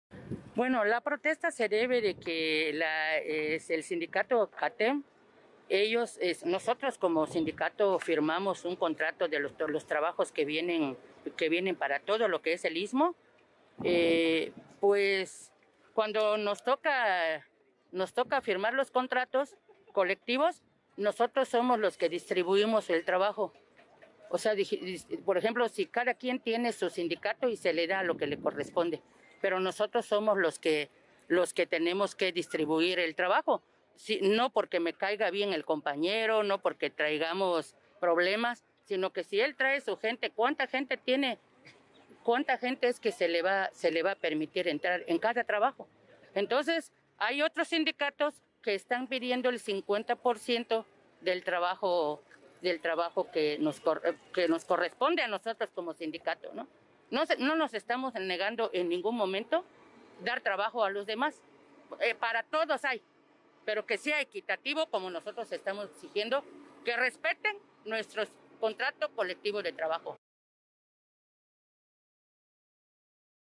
BITE BLOQUEO CATEM
En respuesta a las acciones realizadas por la CTM y el Sindicato Libertad en el istmo, el día de hoy se movilizaron los agremiados al CATEM con bloqueos carreteros en la región del istmo de Tehuantepec, para exigir que se respete la titularidad del Contrato Colectivo de Trabajo, en las obras que se realizan en esta zona de la entidad Oaxaqueña.
El día de hoy las protestas se instalaron a la altura del Canal 33 en Juchitán, en el crucero de La Ventosa, Salina Cruz a la altura de la Agencia de Autos, y el acceso principal, además de la vía estatal que conecta a Unión Hidalgo con la ciudad de Juchitán, en el Caracol de Tehuantepec, y a la altura del Caracol en Ciudad Ixtepec.
La postura que presentan los inconformes, es que el trabajo sea distribuido de manera equitativo entre todos los gremios que han pedido participación en las obras, y que se respete la firma del contrato hecho por la dirigencia del CATEM.